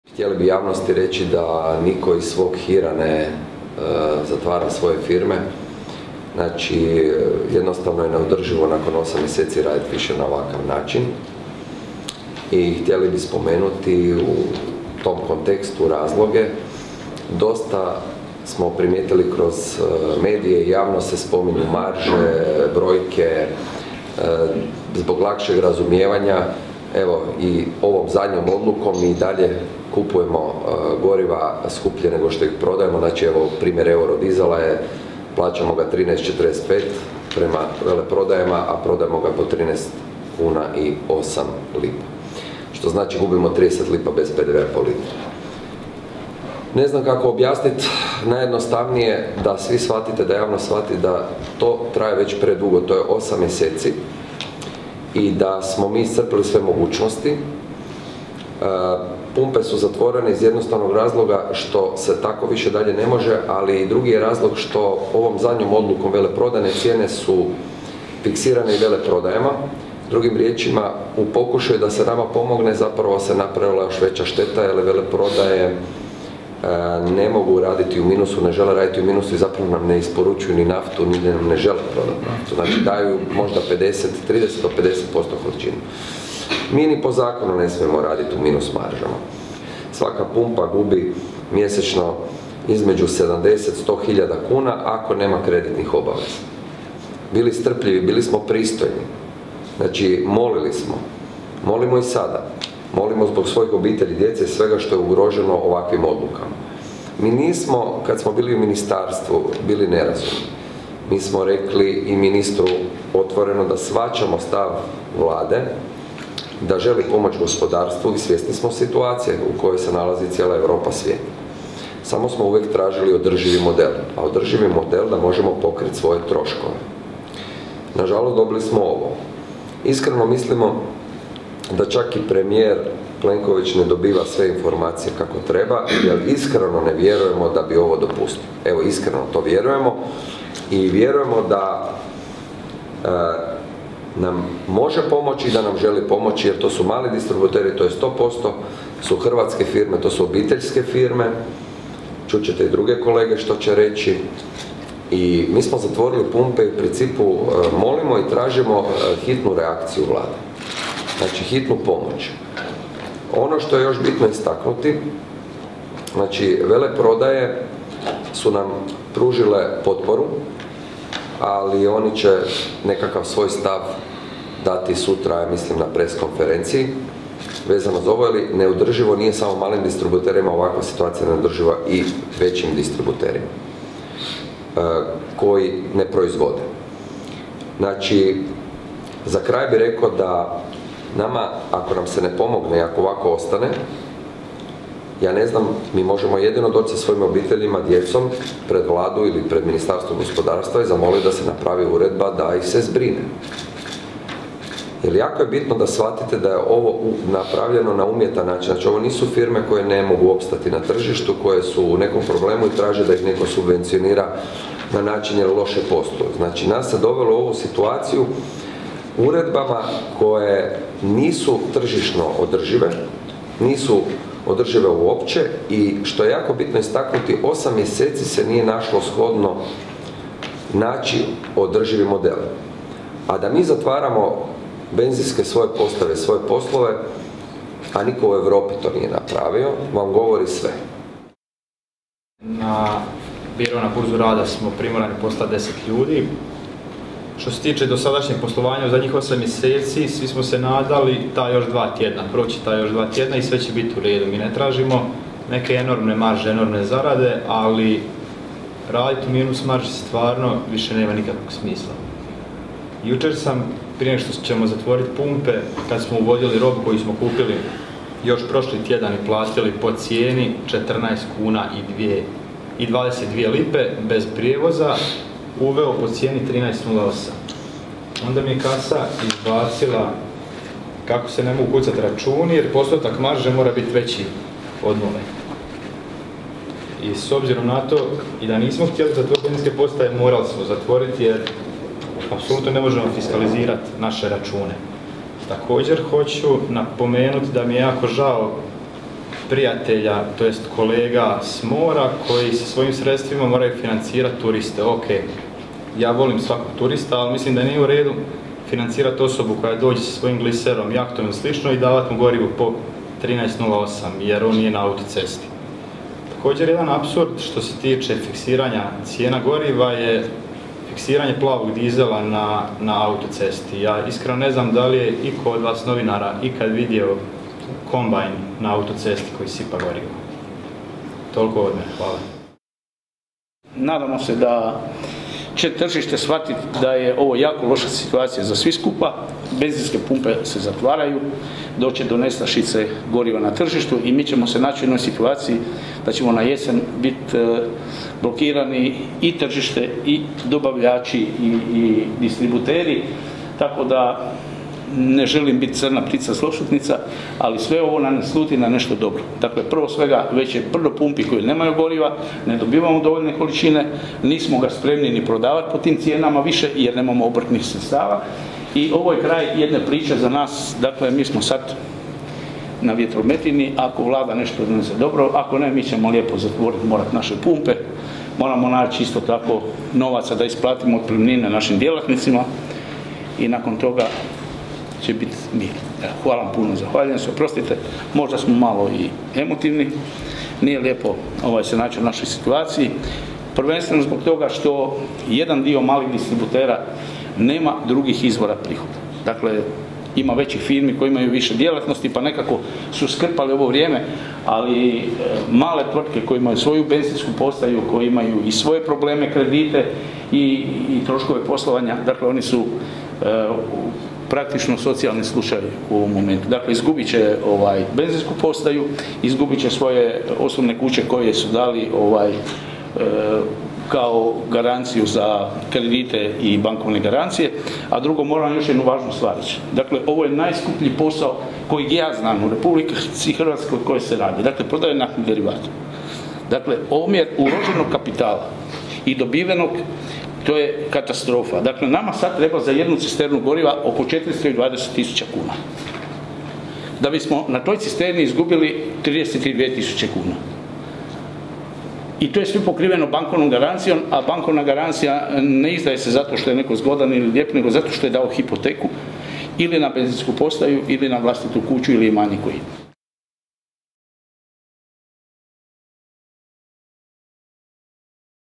Udruga malih distributera goriva o zatvaranju benzinskih postaja
Udruga malih distributera goriva održala je konferenciju za medije povodom Odluke Vlade RH i Uredbe o utvrđivanju najviših maloprodajnih i veleprodajnih cijena goriva.